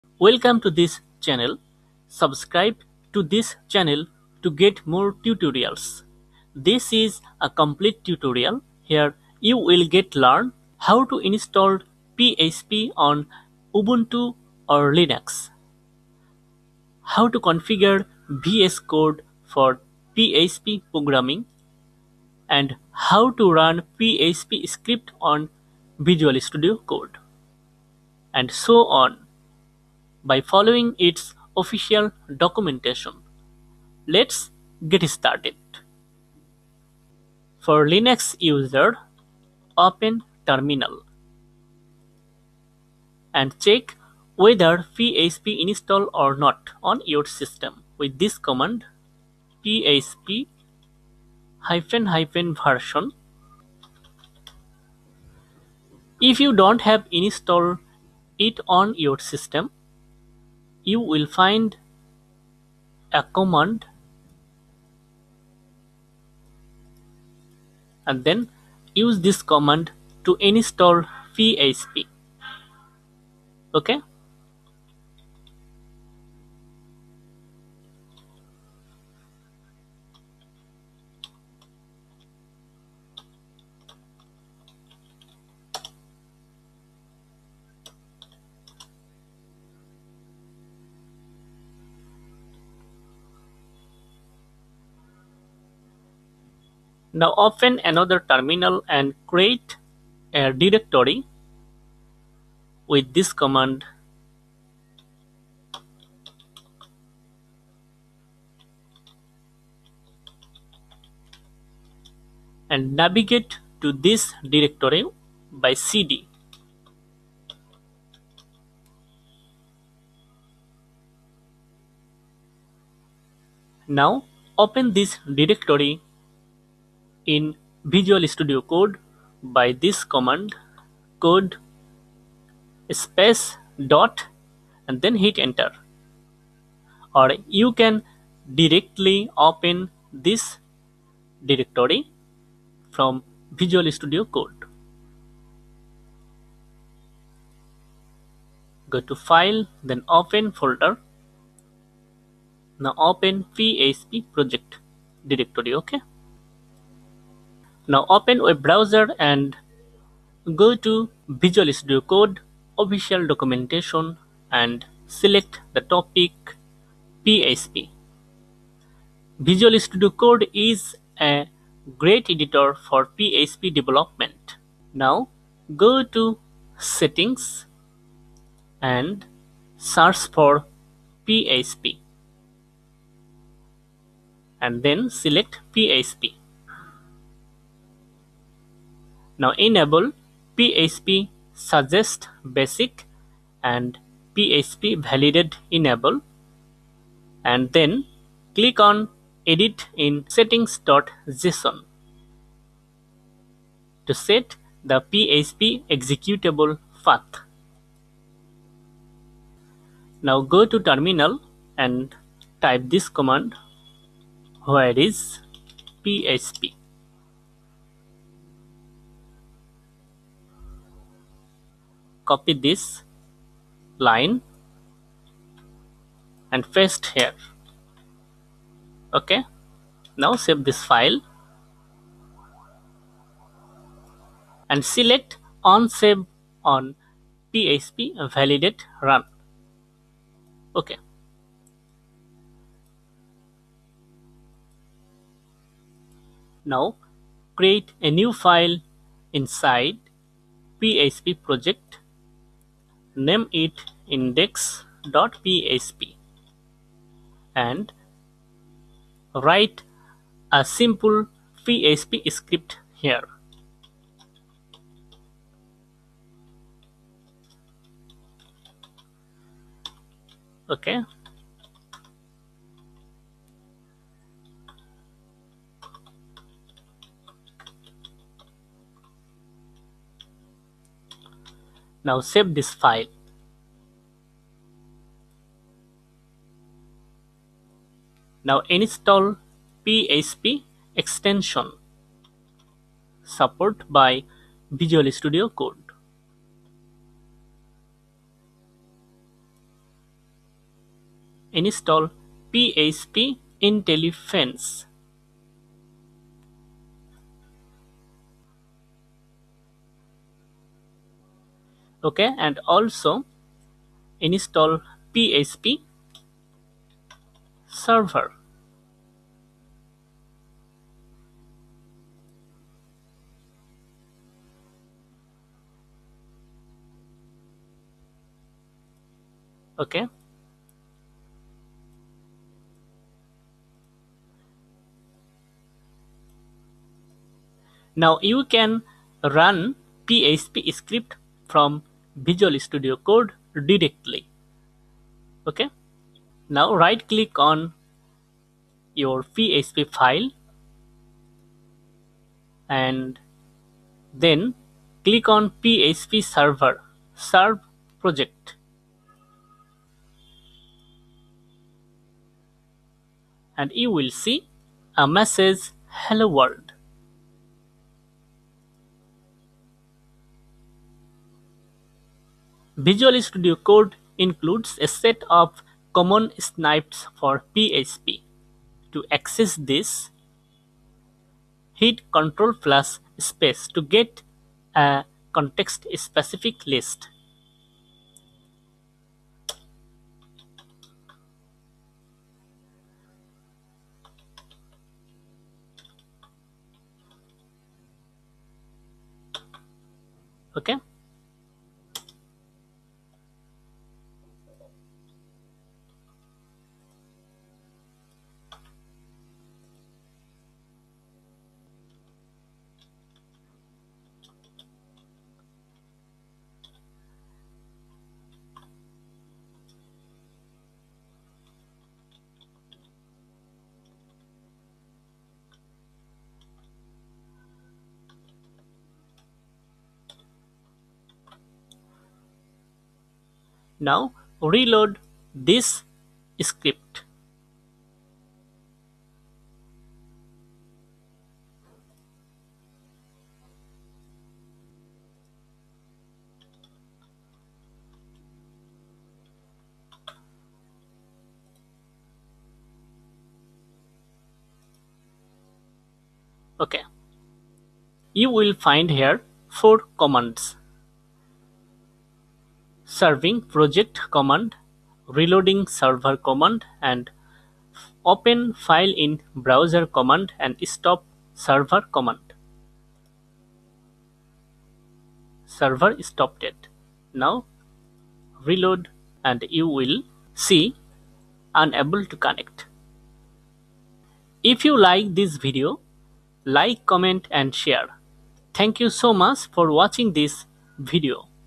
How to config and run PHP on vscode live server | PHP Programming with Visual Studio Code in Linux
This is a complete tutorial, here, You will get learn How to install PHP on Ubuntu 20.04 LTS or Linux and How to configure vscode for PHP Programming Language and also learn How to run PHP script on vscode live server?